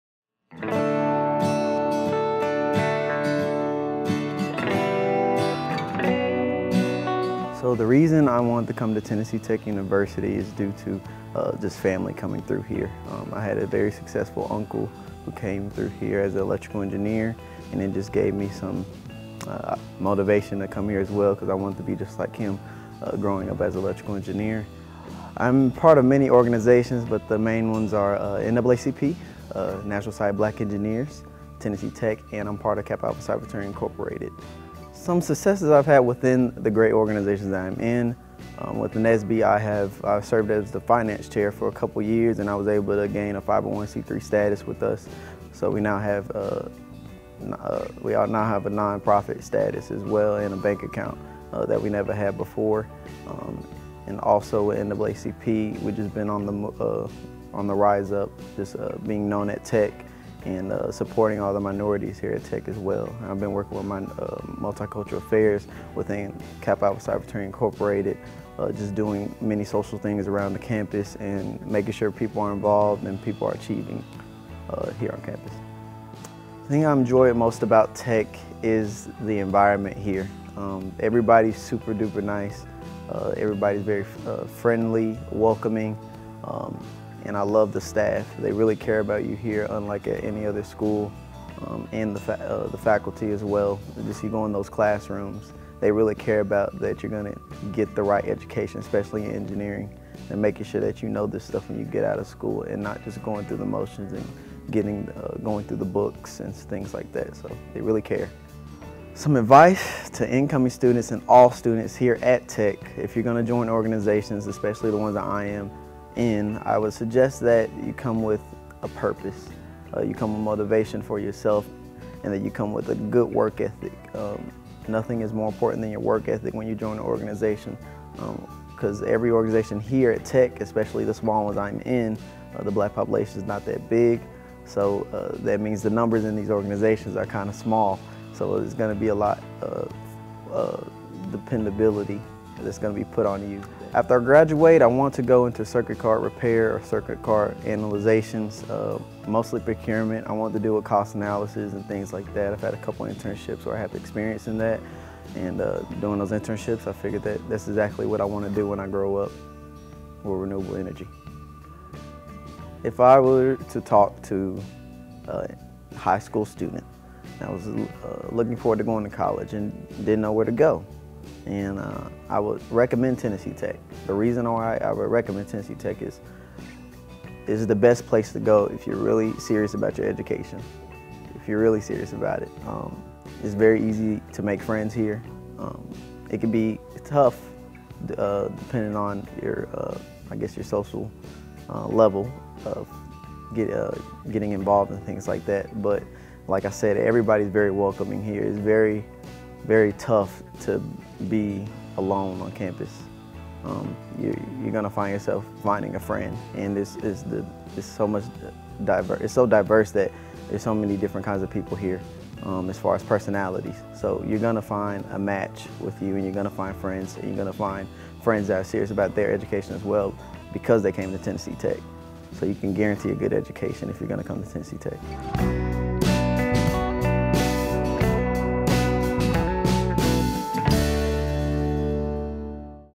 TNTech Stories - Jymon T. Scott
As a young black man, Jymon T. Scott, electrical engineering ’22, came to Tennessee Tech with a purpose. As he was inspired by his uncle to study engineering at Tech, he has also set out to be an inspiration and role model for students of color.